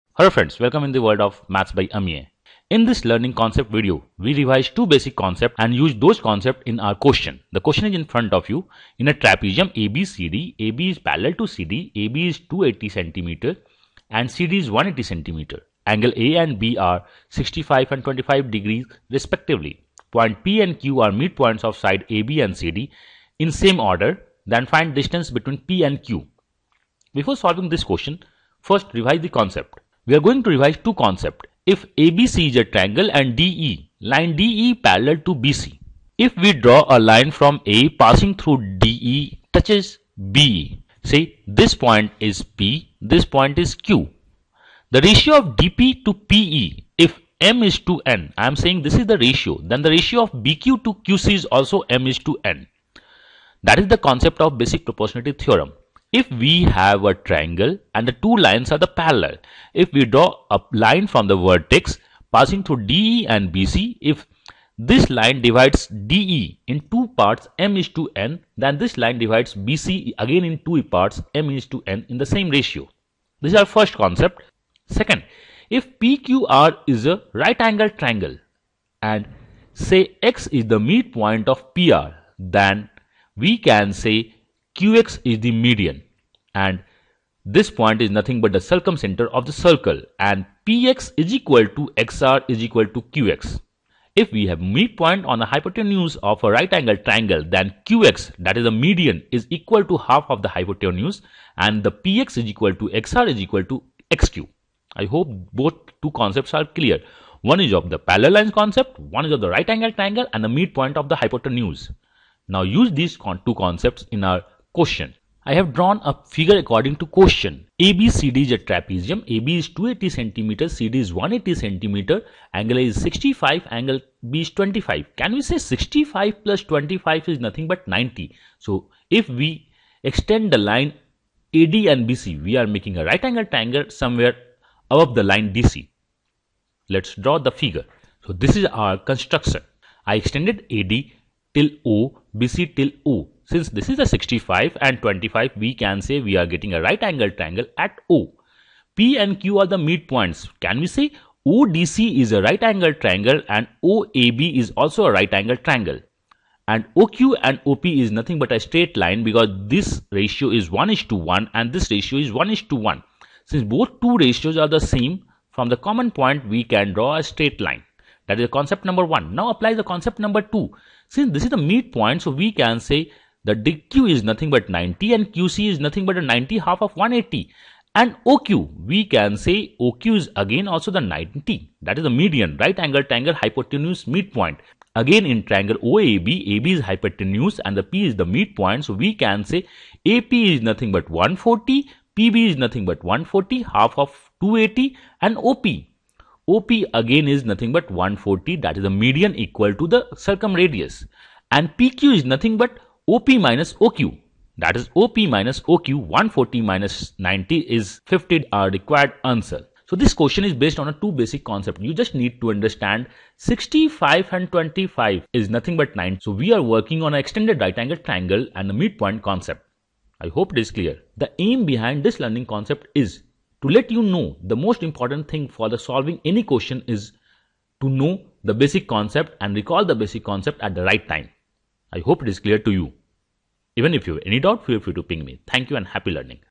Geometry for CAT 2024 : Session 31 : Mid point in trapezium Special case
In a trapezium ABCD , AB||CD, AB=280cm & CD=180 cm. Angle A & B are 65 & 25 degree respectively. Point P & Q are mid points of side AB & CD in same order then find distance between P & Q.